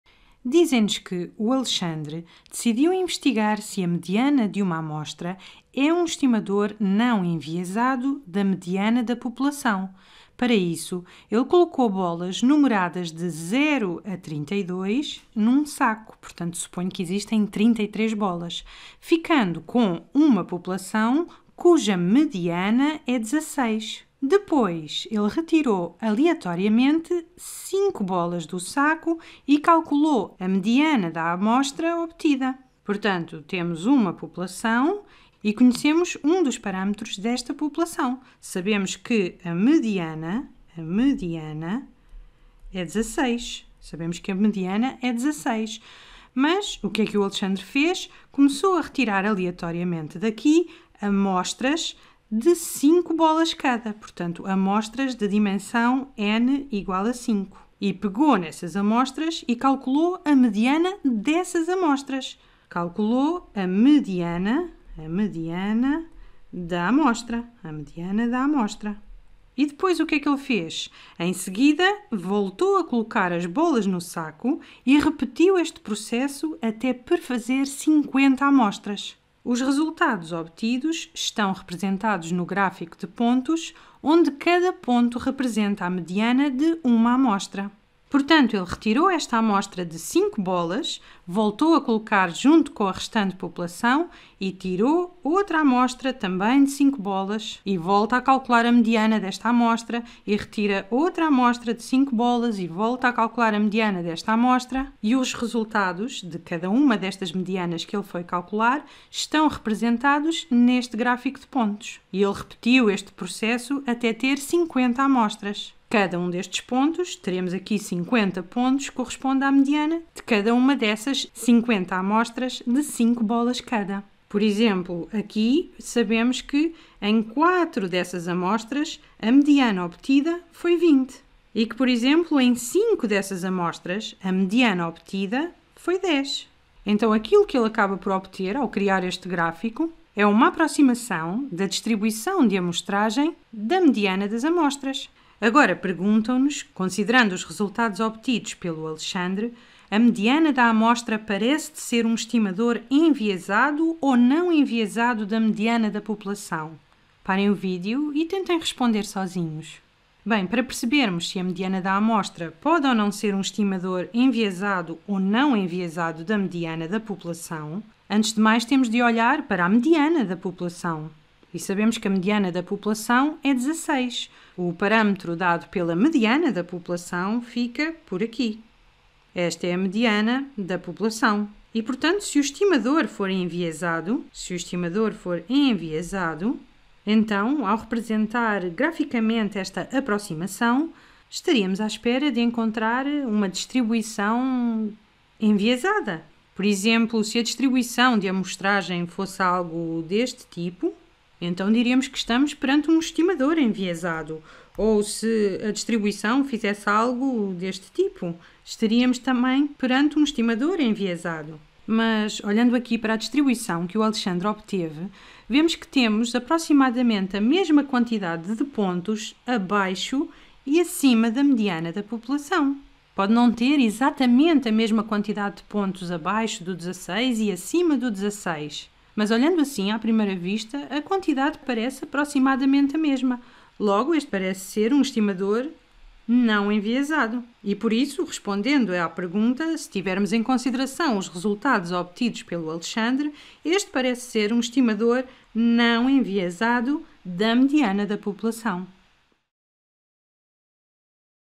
Estimador enviesado ou não enviesado: exemplo - Khan Academy em português (MACS 11.º ano)
Vídeo original: Sample statistic bias worked example